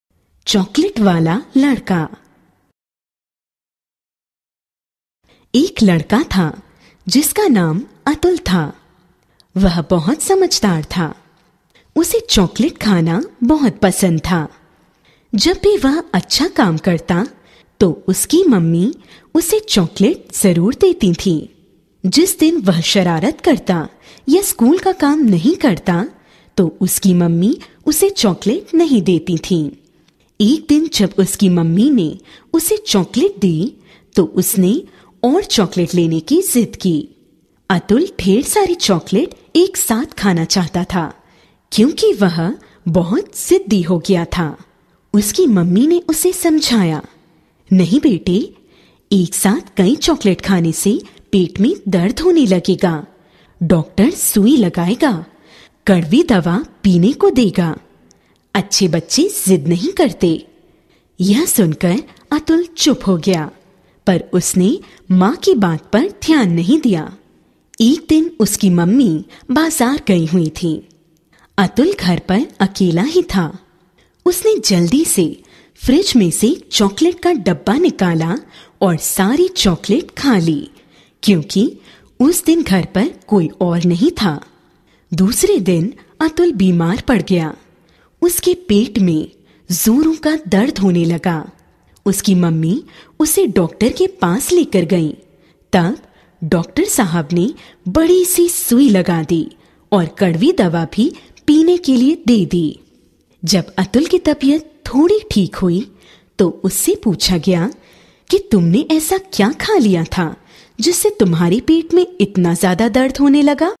Story of a Boy who liked Chocolates
This story is about a boy who always liked chocolates. He was always given a chocolate by his mother when he behaved good and completed his works on time. One day, he was refused chocolates by his mother saying it would disturb his health as he urged to eat a complete box.  As, he was stubborn he didn’t listen to his mother which resulted in ill health.
What happened next and the moral the boy learned for not listening to his mother has to be viewed in the above video.
This animated video would be a good lesson to the children as it is colourfully animated and presented.